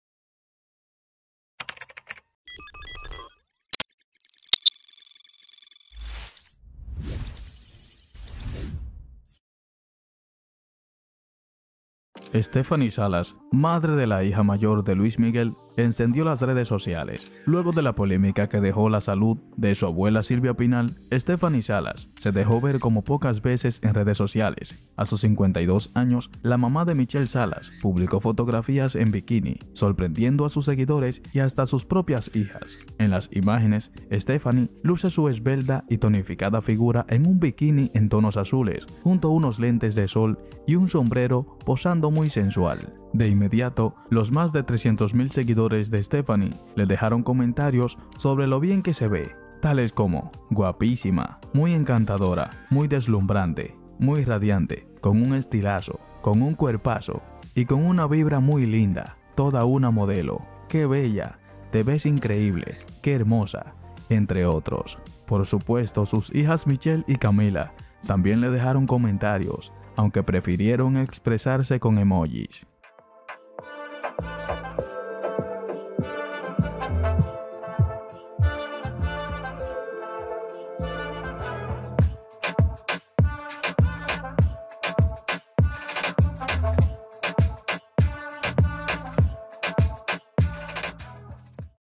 Stephanie Salas luce en bikini a sus 52 años
A sus 52 años, la mamá de Michelle Salas publicó fotografías en bikini, sorprendiendo a sus seguidores y hasta a sus propias hijas.